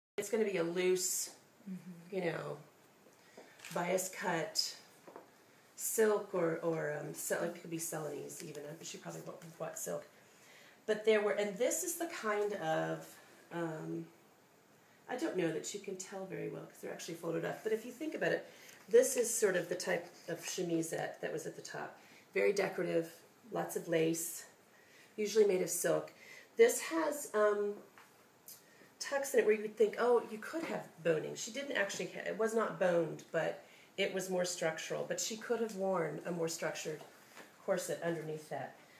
Textile Conservator Claudia Walpole describes beaded gown part 1
Behind the scenes with Claudia Walpole, Textile Conservator, as she prepares for "Star Power: Edward Steichen's Glamour Photography," on view at Reynolda House Museum of American Art, 2/23/13 through 5/19/13.

ReynoldaHouse.org